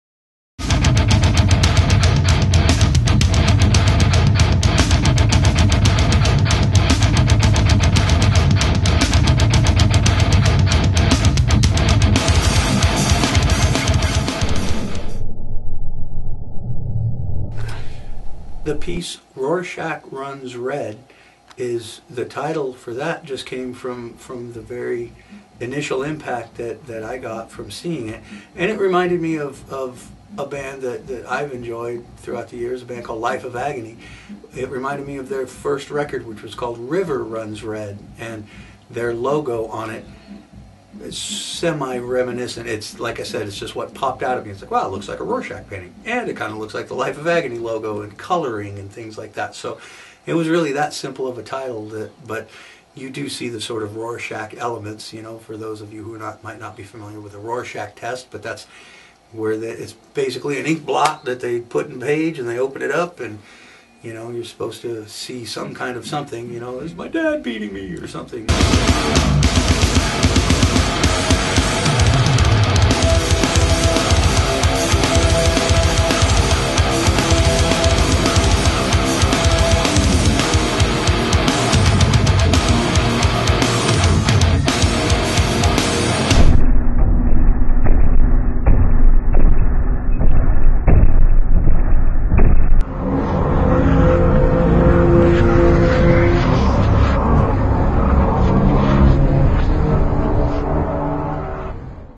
Gene Hoglan discusses his art piece "Rorschach Runs Red,"
New Video - Gene Hoglan discusses his art piece "Rorschach Runs Red," part of his collection of artwork made from rhythm.